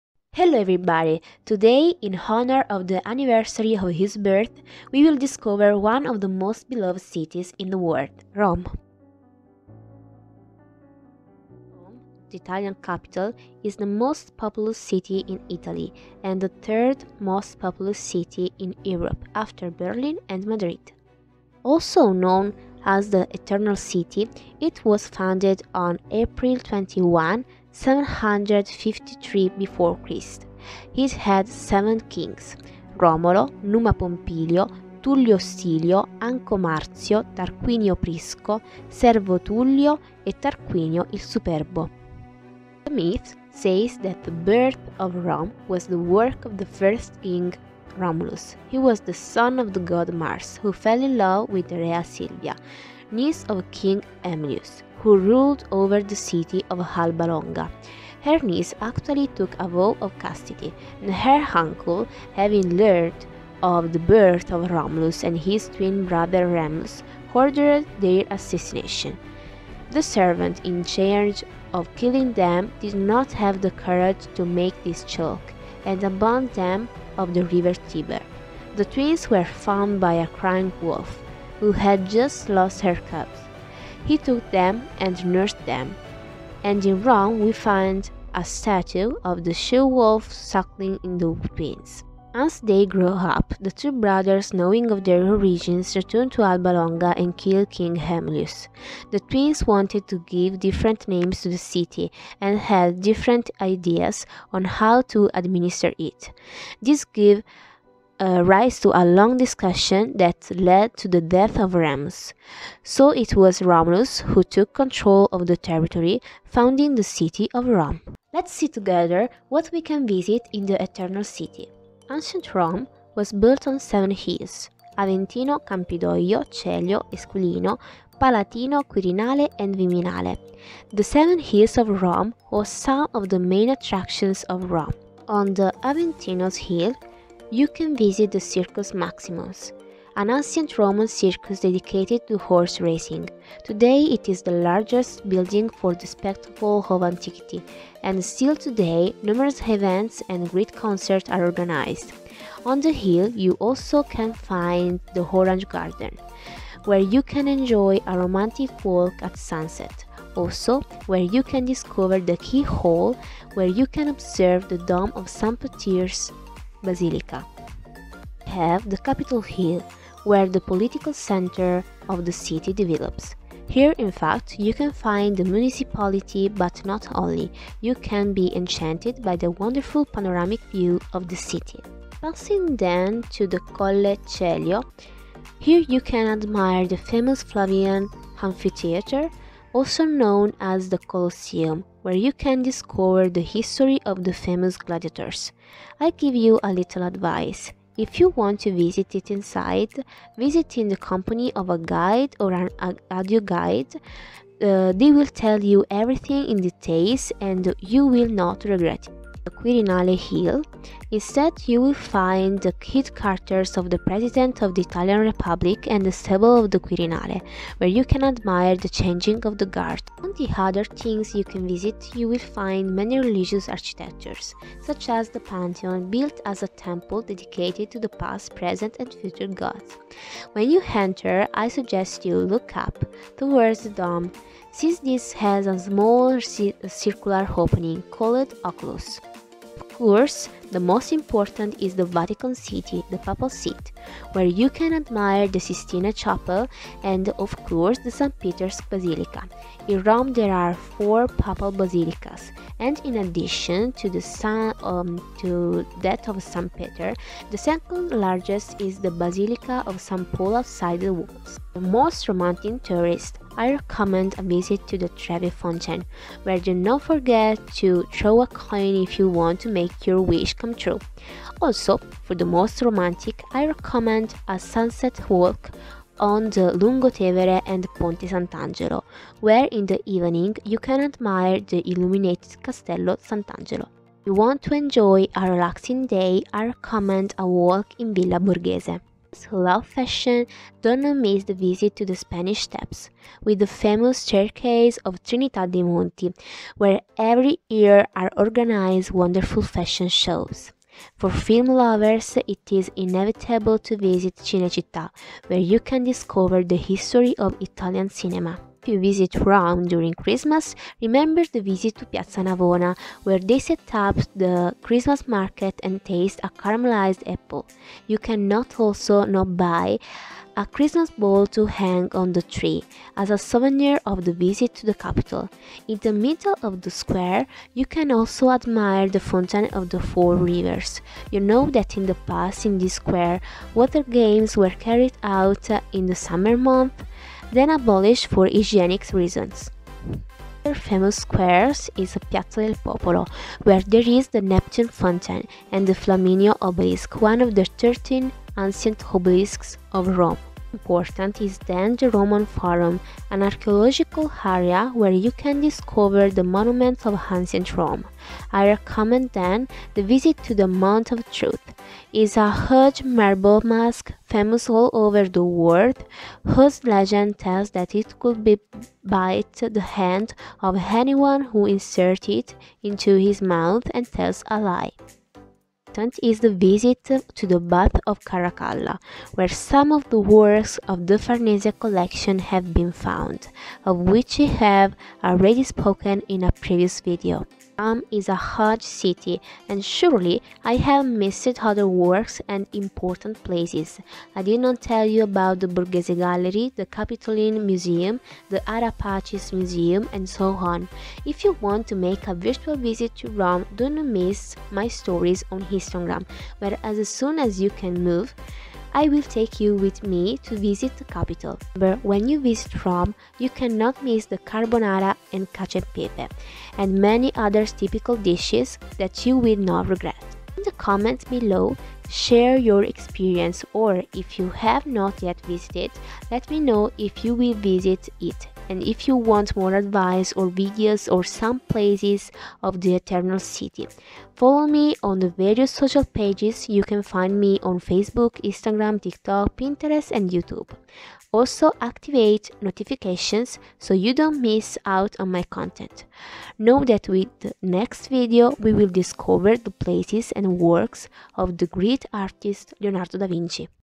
Rome, the Eternal City!
With this video I want to celebrate the anniversary of one of the most beautiful cities in Italy: Rome!

Voice of Gaia Iavarone
Videomaker: Gaia Iavarone
Photos: web source